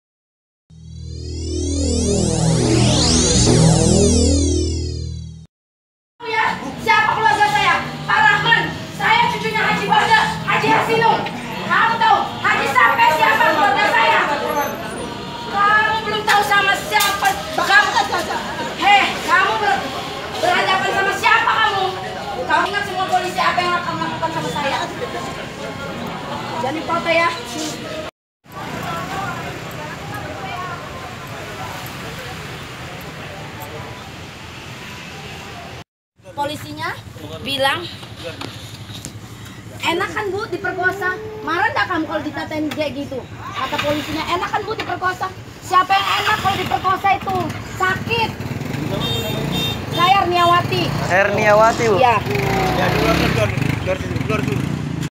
VIDEO Mengaku Dirudapaksa Oknum Aparat Wanita ini Berteriak di Penjagaan Kantor Polisi
Ingin berita lainnya silahkan kunjungi di bawah ini